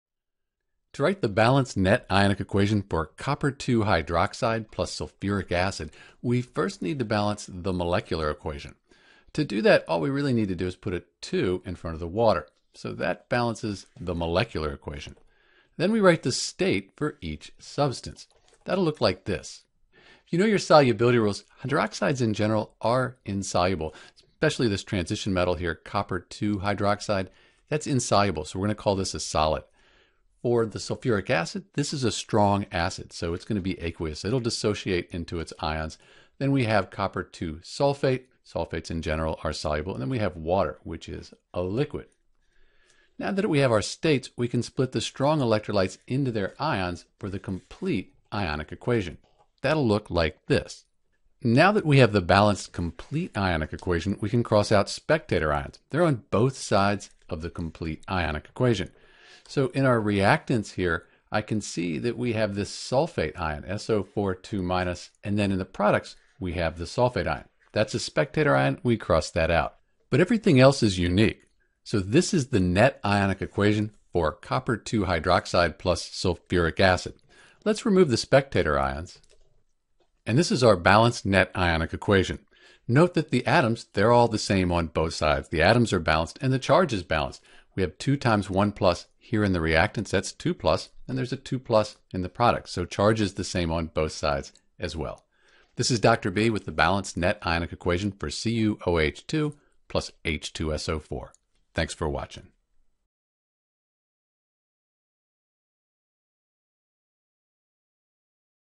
How to Write the Net Ionic Equation for Cu(OH)2 + H2SO4 = CuSO4 + H2O
There are three main steps for writing the net ionic equation for Cu(OH)2 + H2SO4 = CuSO4 + H2O (Copper (II) hydroxide + Sulfuric acid). First, we balance the molecular equation. Second, we write the states and break the soluble ionic compounds into their ions (these are the strong electrolytes with an (aq) after them). Finally, we cross out any spectator ions. These are the ions that appear on both sides of the ionic equation.

More Practice

General Steps:
1. Write the balanced molecular equation.
2. Write the state (s, l, g, aq) for each substance.
3. Split soluble compounds into ions (the complete ionic equation).
4. Cross out the spectator ions on both sides of complete ionic equation.
5. Write the remaining substances as the net ionic equation.